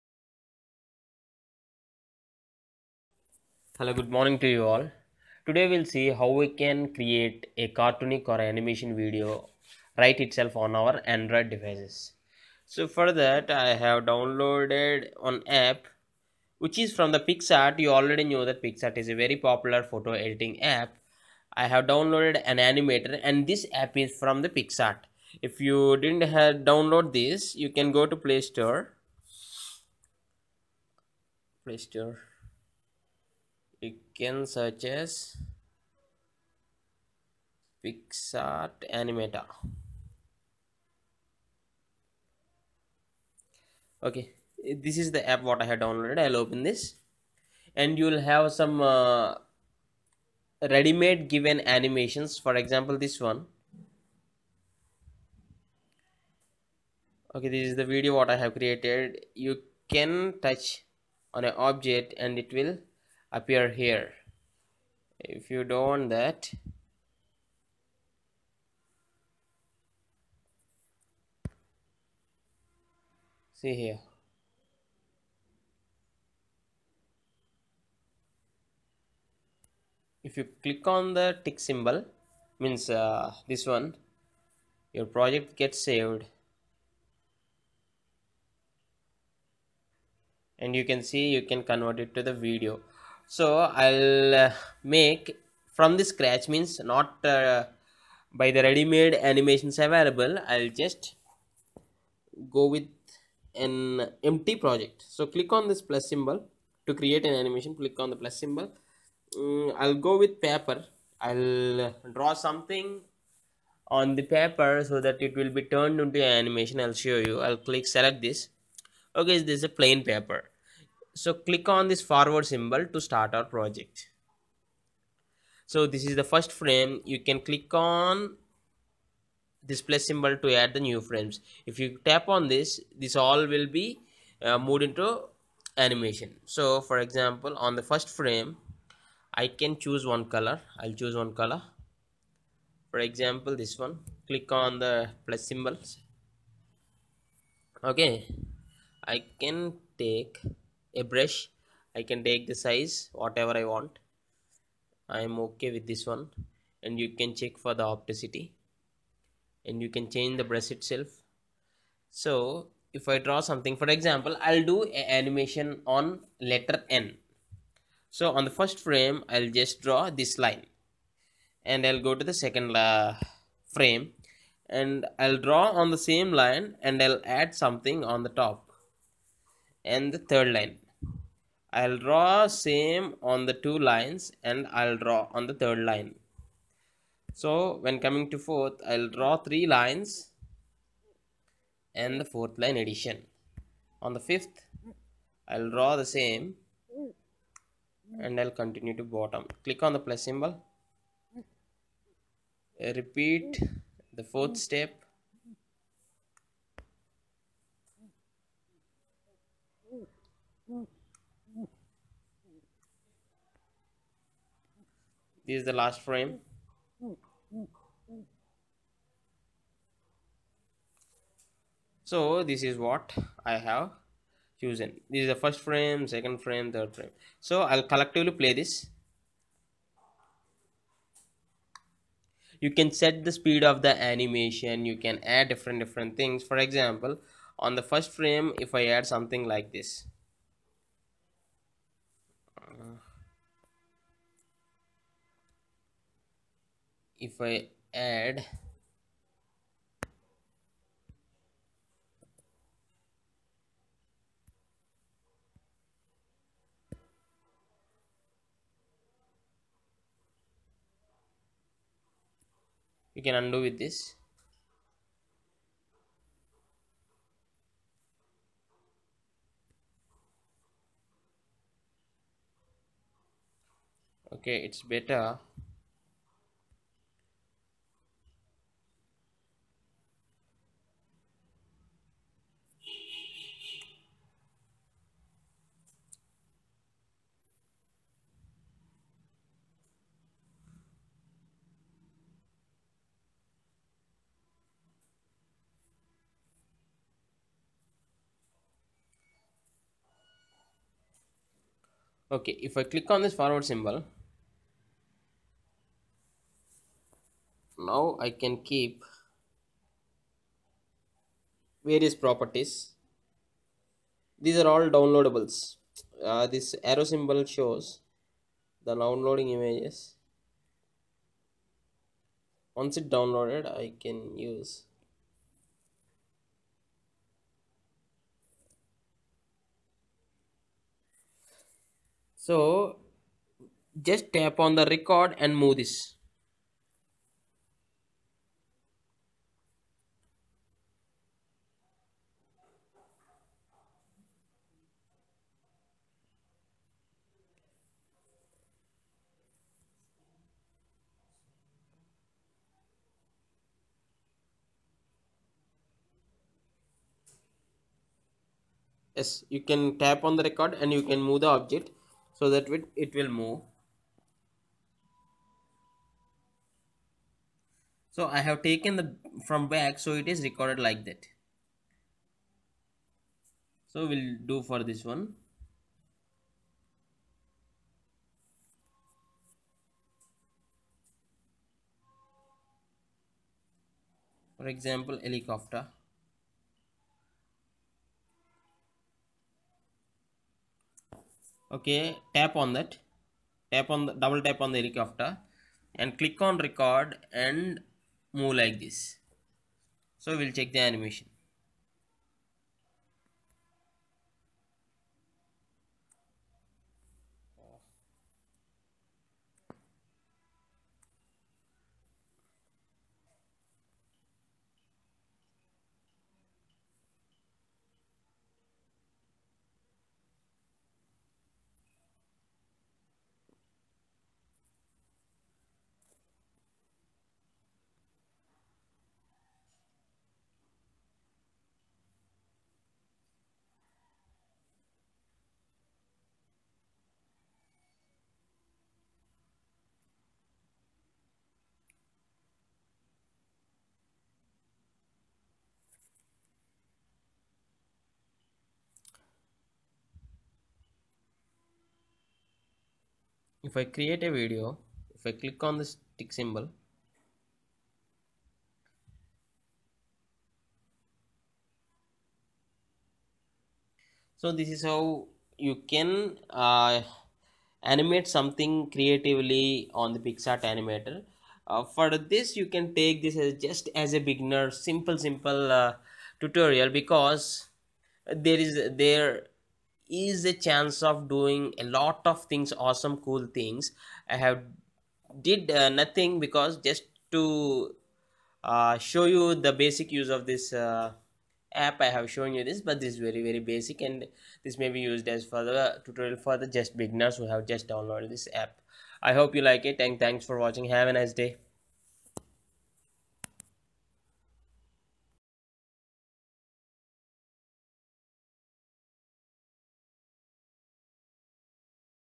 How to create animation in android using Picsart animator app
How to create animation in right in android mobile phone using the picsart animation app,yes with picsart animation app we can easily do animation in android mobiles see how  and watch this tutorial video on picsart animation android app.